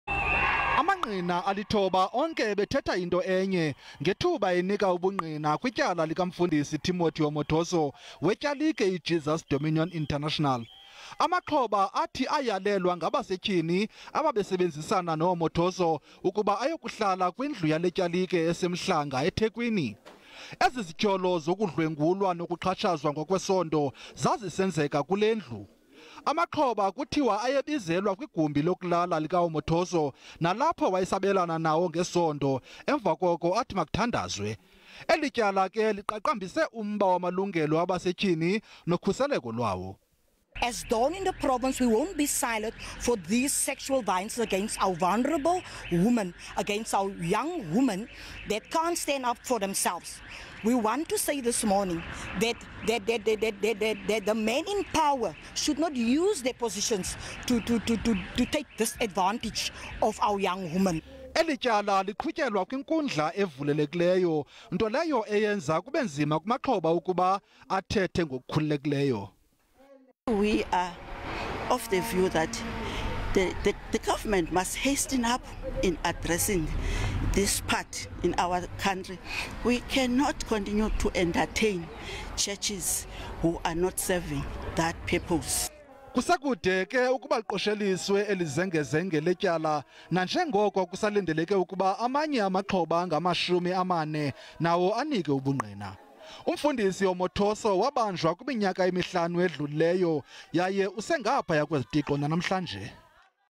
Lisaqhuba ityala lika Omotoso
Lisaqhuba ityala likaTimothy Omotoso  kwinkundla ephakamileyo yaseBhayi. Kuleveki siphuma kuyo mabini amaxhoba athe anika ubungqina kulenkundla. Ukuzakuthi ga ngoku alithoba amantombazana athe anika ubungqina kwizityholo zokuxhatshazwa ngokwesondo ngulo ka Omotoso.

Ukufumana ezinye iindaba, ndwendwela i-sabcnews.com, SABCNews SABCIindaba GBV kumajelo onxibelelwano.